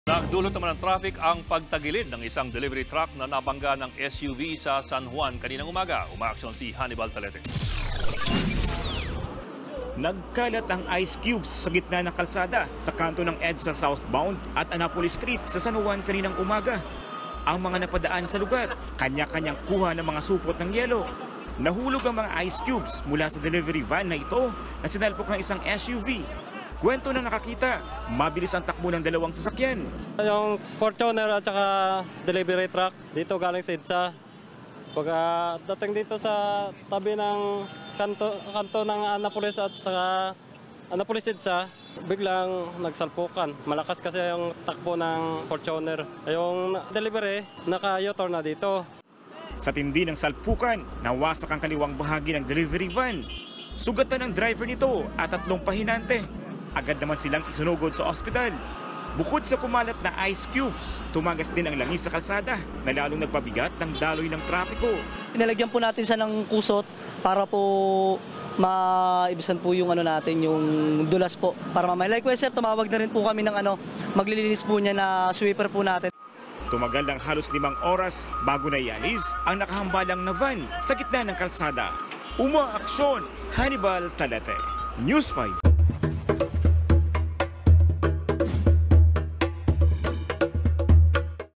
Ice cubes, nagkalat sa EDSA
Tumambak ang mga yelo na dala ng delivery van matapos makabanggaan ang isang SUV. | via Hannibal Talete